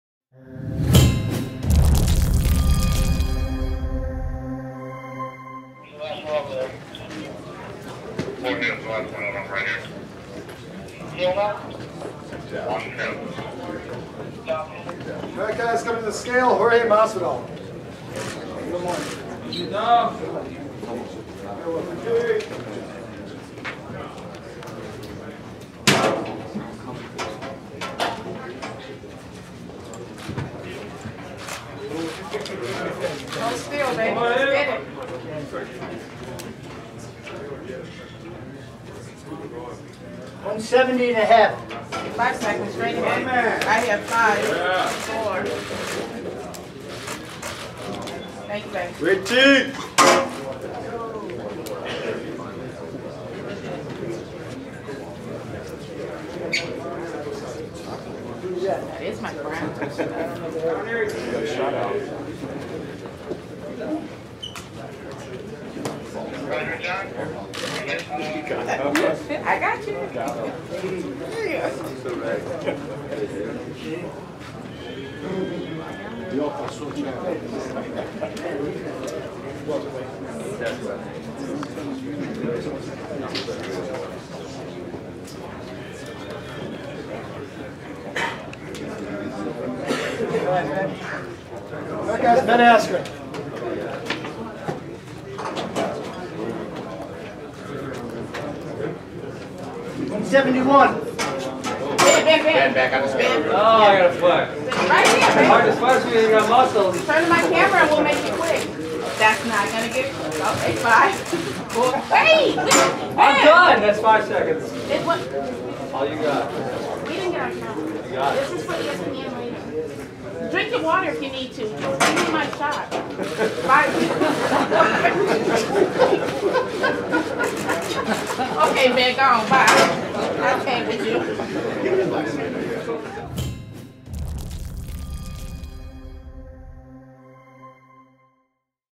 UFC 239 Official Weigh: Ben Askren vs. Jorge Masvidal
See Ben Askren and Jorge Masvidal weigh in at the UFC 239 Official Weigh-Ins.